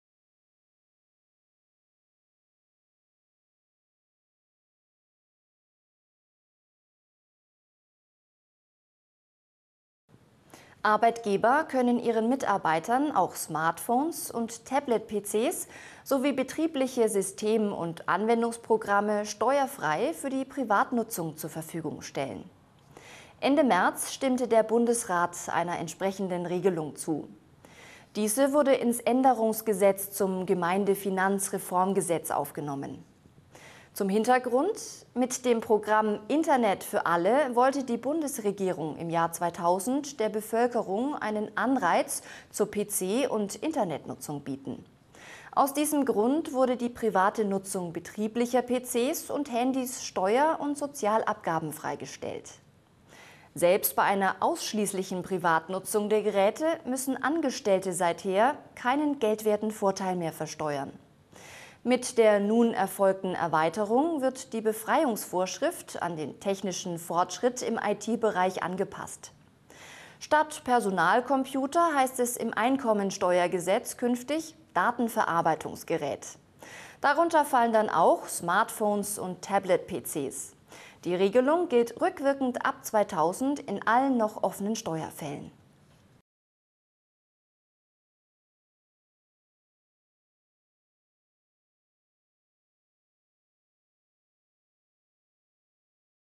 NWB Steuer-TV | Steuerfreie Überlassung von Smartphone und Tablet-PC
Arbeitgeber können ihren Mitarbeitern auch Smartphones und Tablet-PCs sowie betriebliche System- und Anwendungsprogramme steuerfrei für die Privatnutzung zur Verfügung stellen. Ende März stimmte der Bundesrat einer entsprechenden Regelung zu...